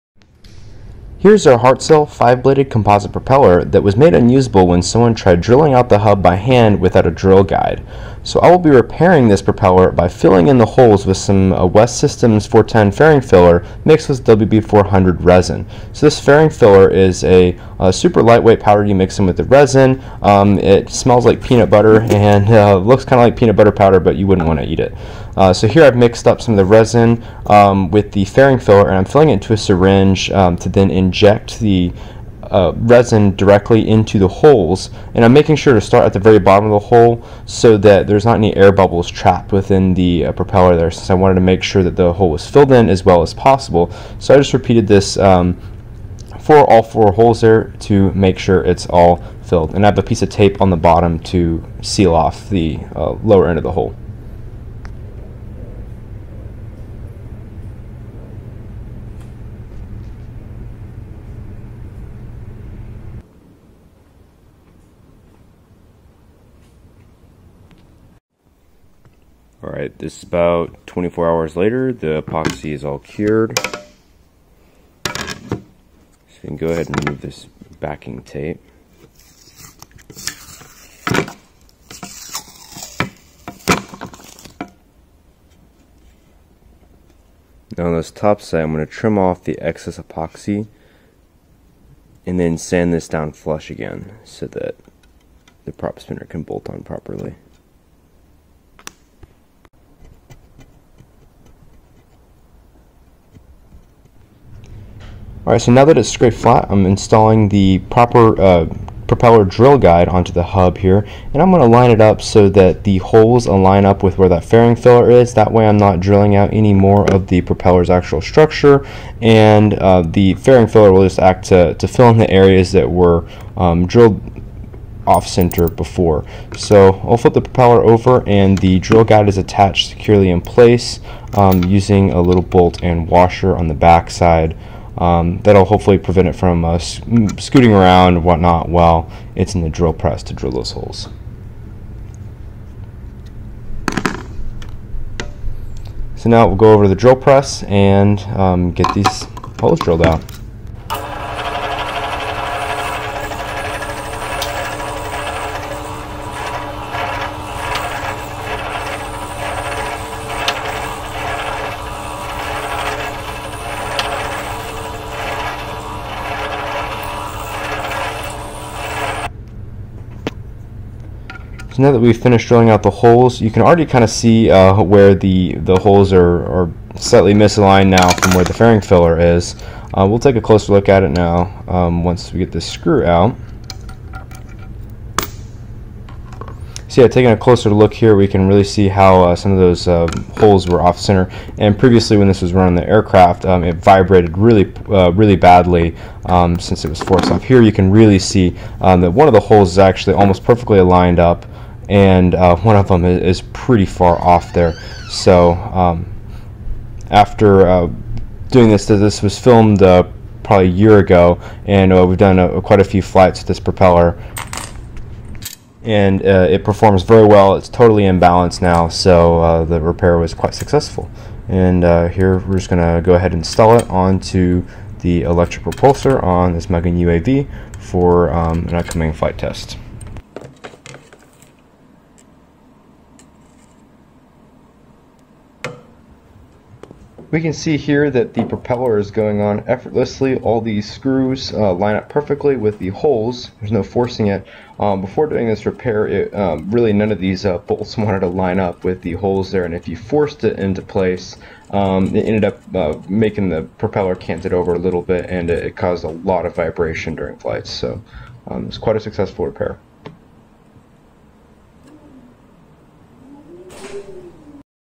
Propeller Hub Mount Hole Repair (Hartzell 5-blade UAV Propeller)
This composite UAV propeller had poorly drilled mounting holes in the hub causing bad vibrations in flight. This repair consists of filling in the holes with an epoxy resin fairing filler then drilling out the hub holes with a proper drill guide. The repair was quite successful and prevented the trashing of an almost brand new propeller.